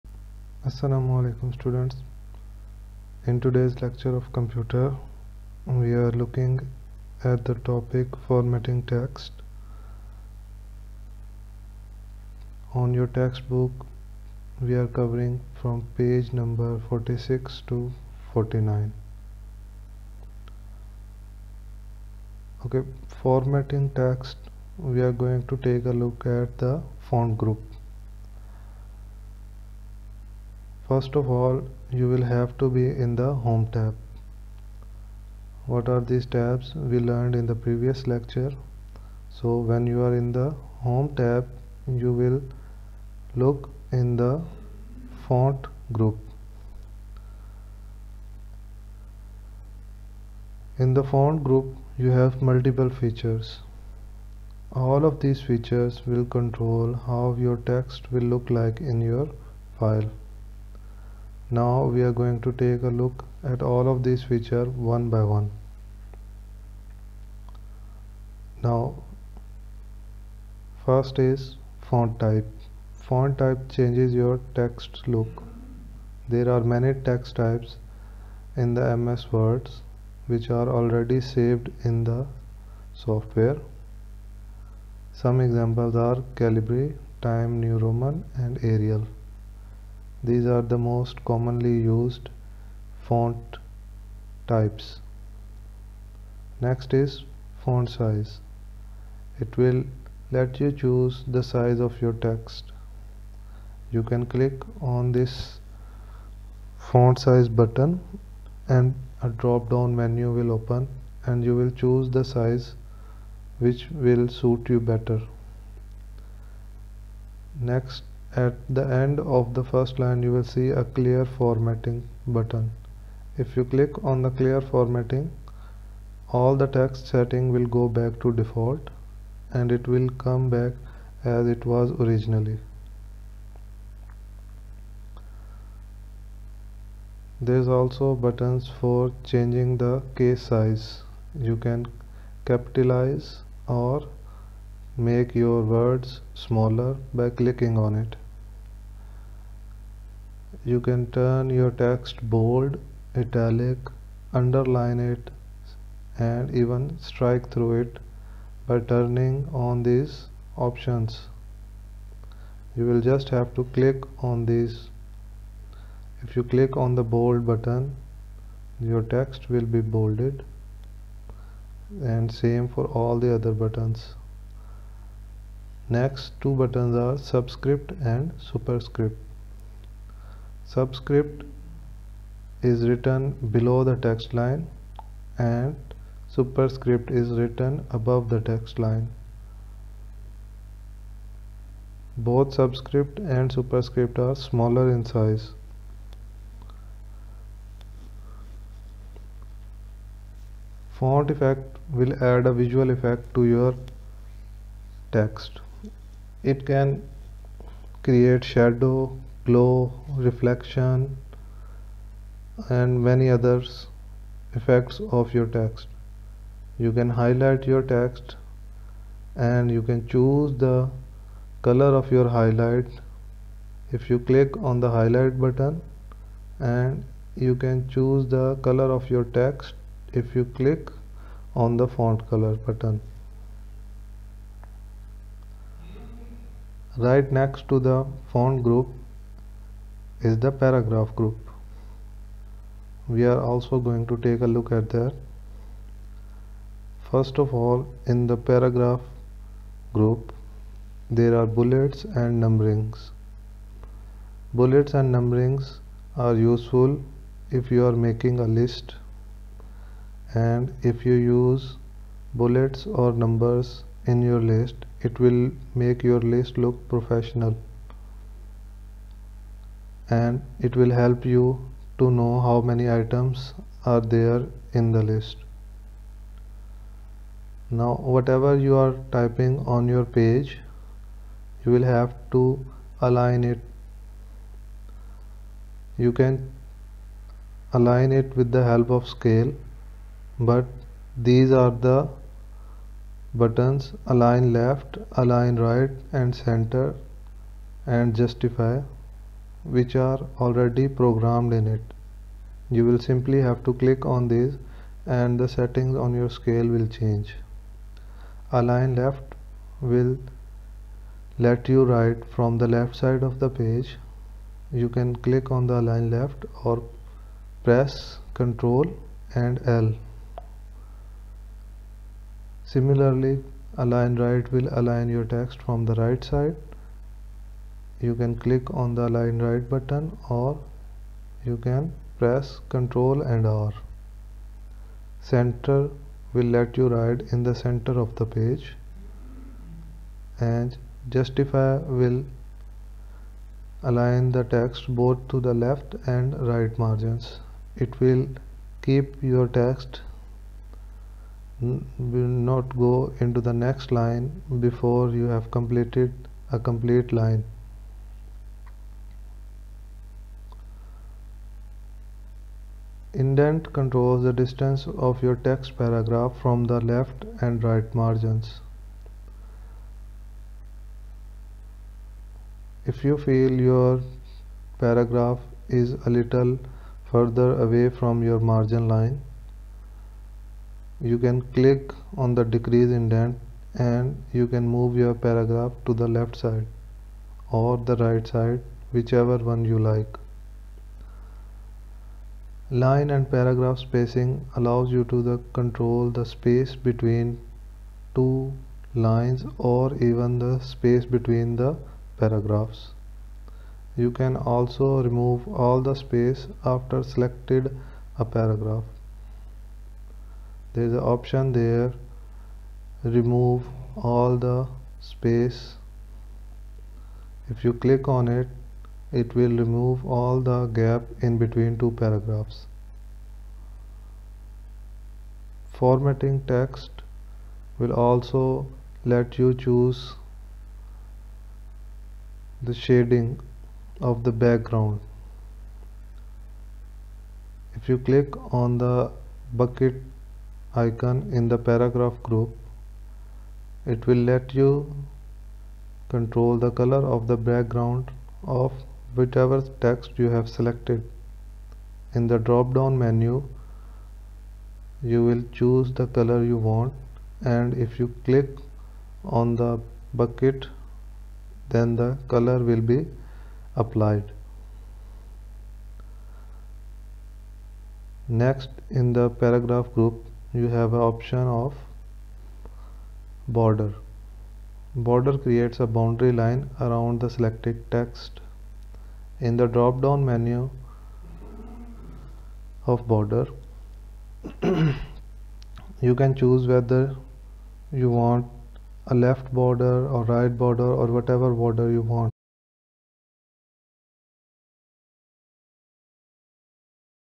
APSACS - OSP - Week 4 - Class 6 - Computer - Lecture 2
This video lecture was delivered online to students during week 4 of Online Support Program.